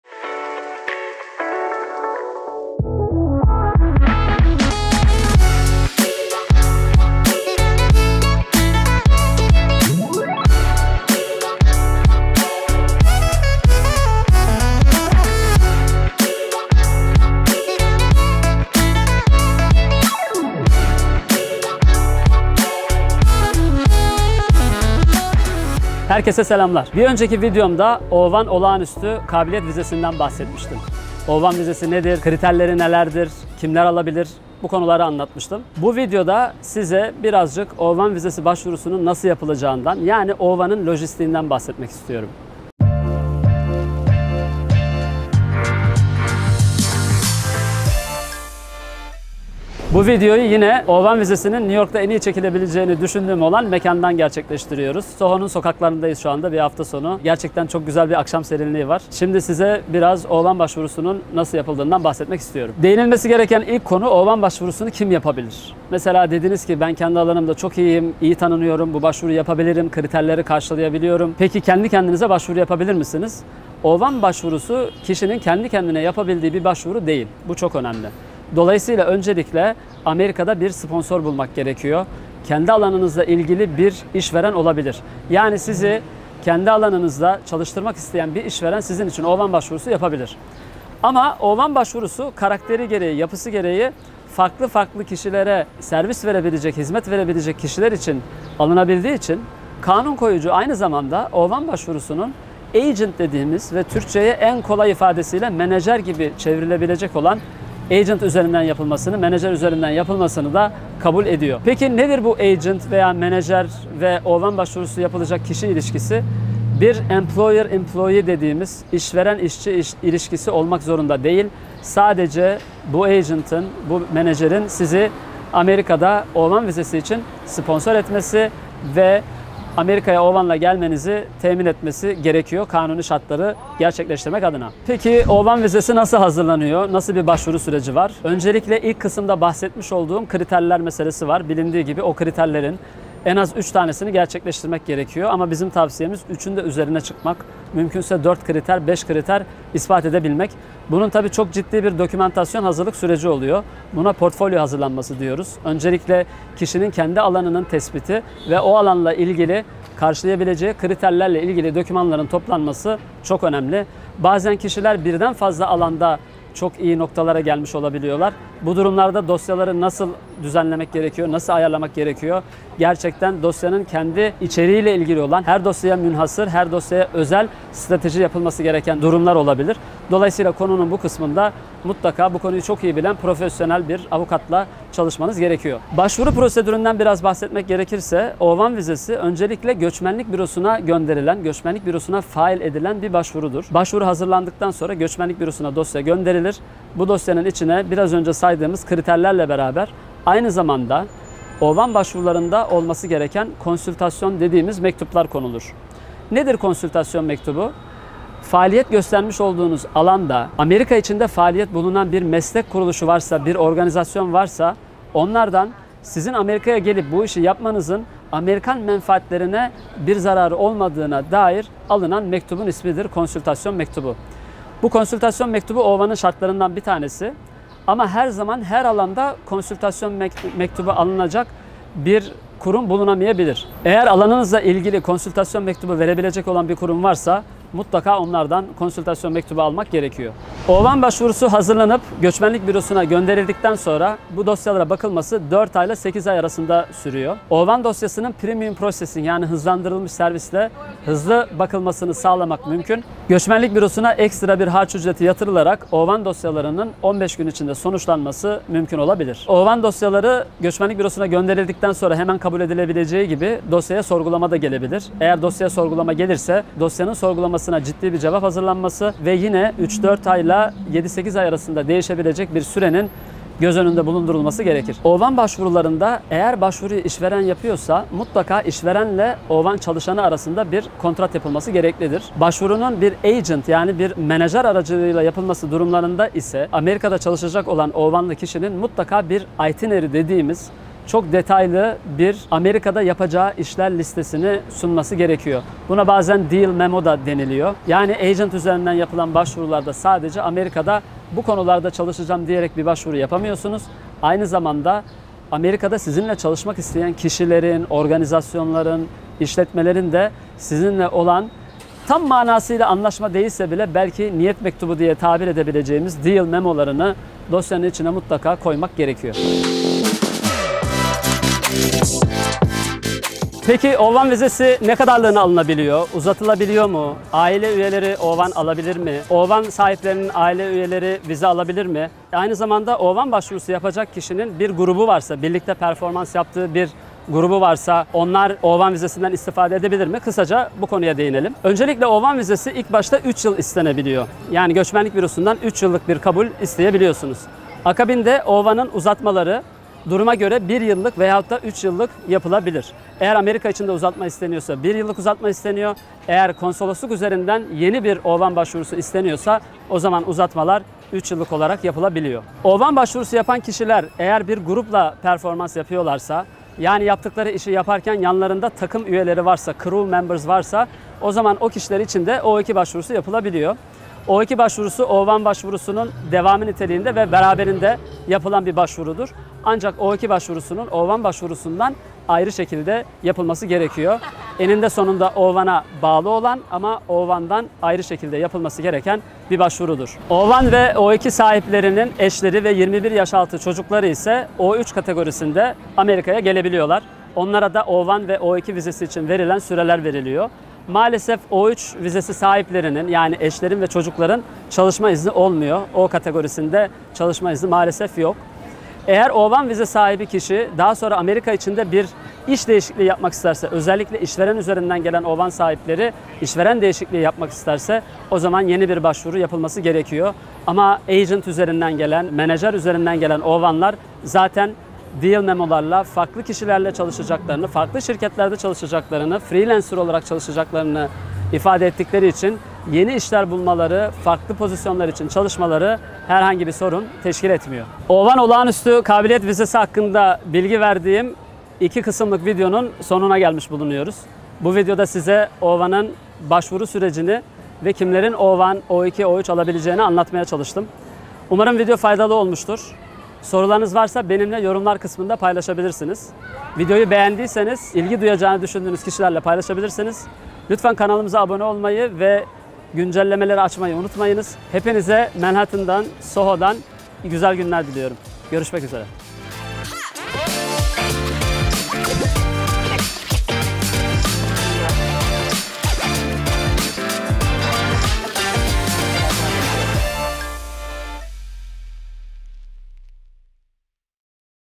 O-1 Vizesi Başvuru Şartları
O-1 başvurusunu kim yapabilir, nereye yapılır, başvuru için neler gereklidir, başvuru süresi nedir, ne kadar süreliğine verilir, O-1 vizesi başvuru sahibinin ailesinin hakları nelerdir?